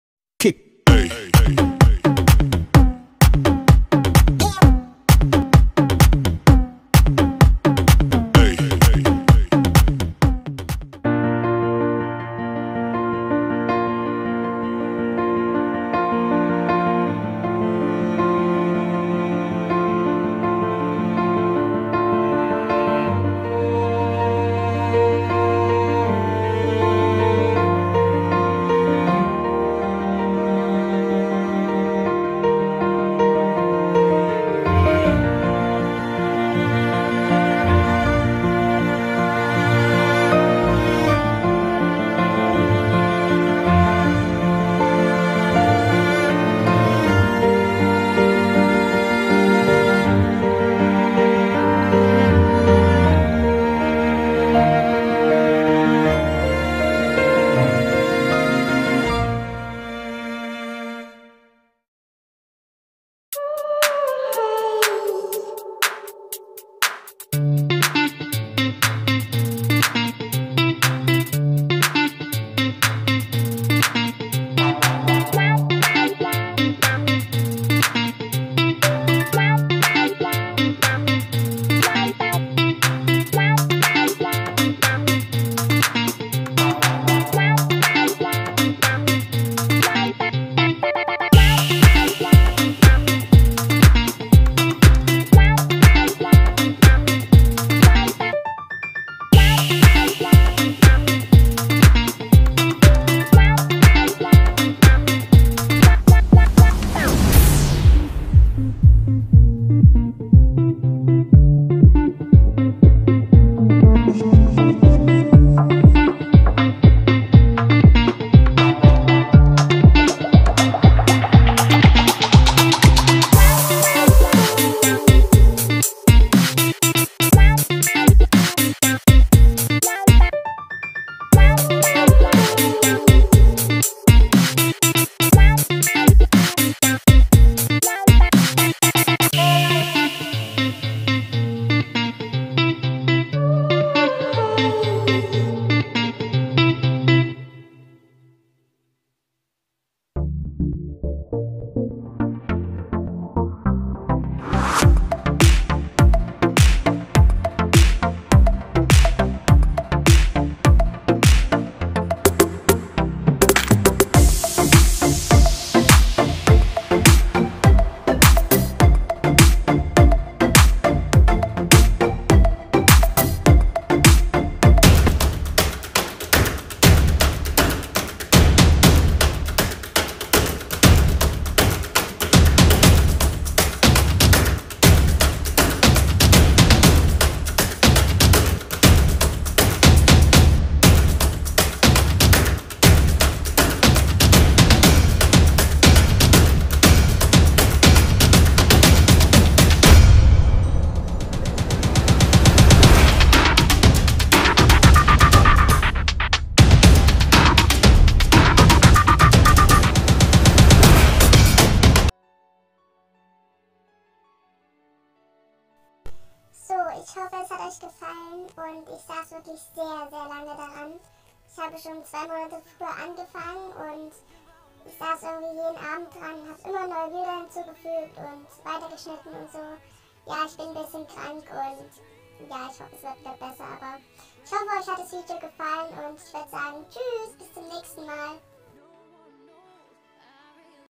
Alles gute maggy!❤❤❤❤🌈🐕
Ihr geburtstag! Am 09.09 endlich ist das video da es hat sooo lange gedauert.

Habe heute keine besondere lust hier groß etwas hinzuschreiben da ich krank bin, hoffe es gefällt euch trotzdem!!❤❤❤